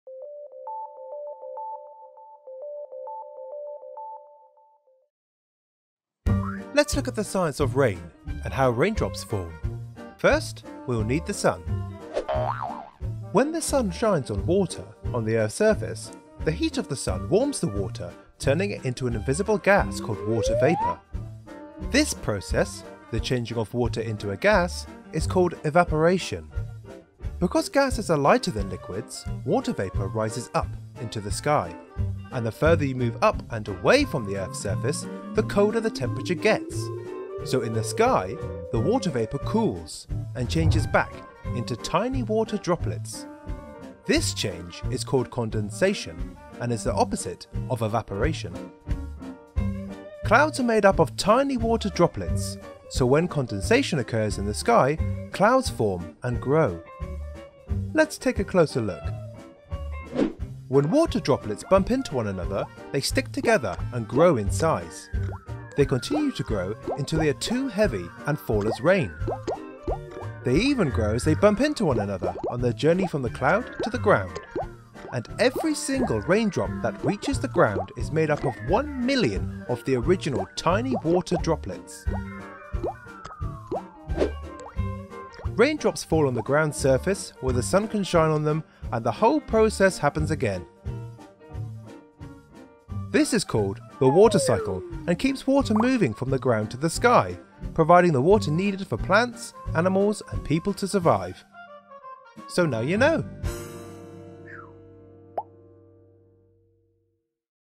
How does rain form and what is the water cycle?
How does rain form? What is the water cycle?

This animated video explains how rain forms and explains how rainfall,  evaporation and condensation all form part of the water cycle.